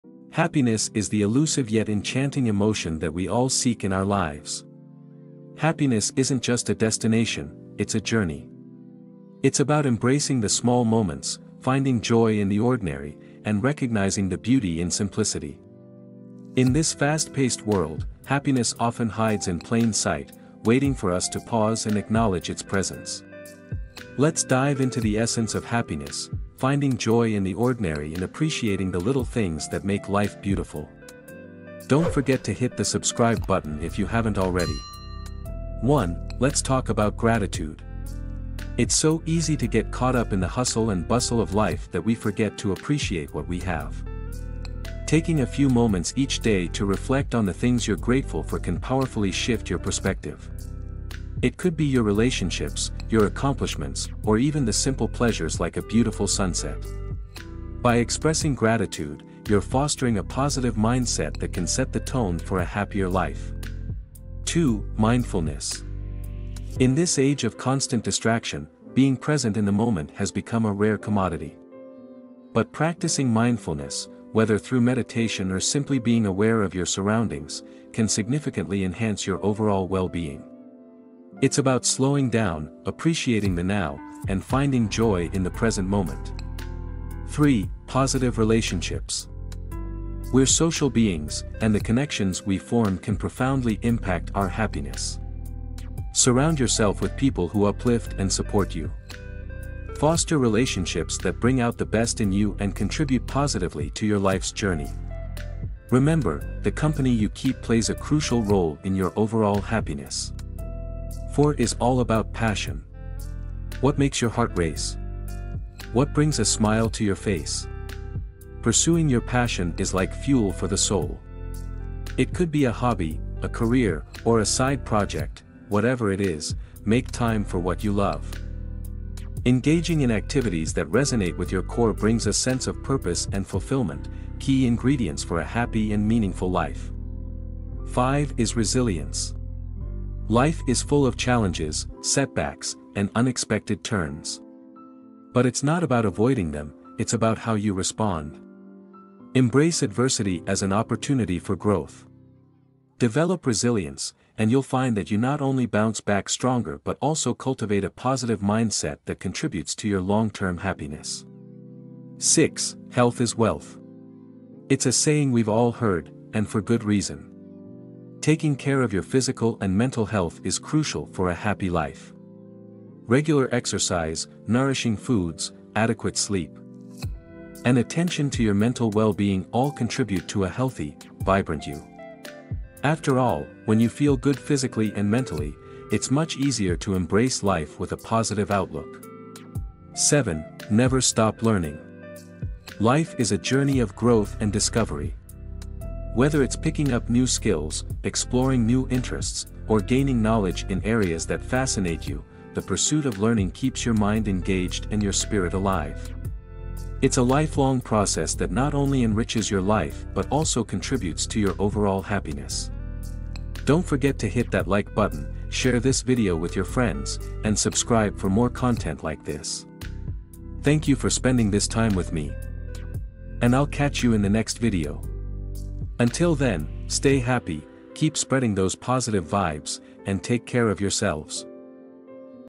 Master the Art of Happiness:Master the Art of Happiness: Learn the 7 Essential Rules for Life
In this video, we're going to be talking about the Master the Art of Happiness: Learn the 7 Essential Rules for Life.

Happiness is an important topic, and it's something that we should all be striving for. In this video, we're going to be discussing the seven essential rules for life, and how we can learn to live by them. From making better decisions to setting realistic goals, these rules will help you to live a happy and fulfilling life. So be sure to watch this video and learn the secrets to happiness!